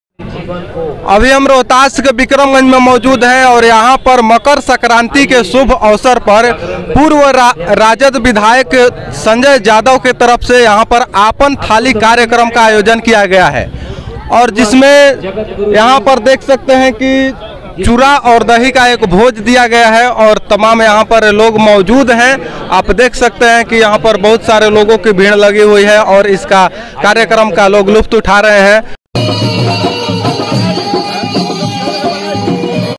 बिक्रमगंज मे भोजपुरी कलाकारों अरविन्द अकेला(कल्लू जी),शिल्पी राज,नीलकमल,ब्रजेश सिंह एव अन्य का आगमन
बिक्रमगंज मे भोजपुरी कलाकारों अरविन्द अकेला(कल्लू जी),शिल्पी राज,नीलकमल,ब्रजेश सिंह एव अन्य का आगमन mediaexpress bikramganj me kallu silpiraj ka stej program
bikramganj ka program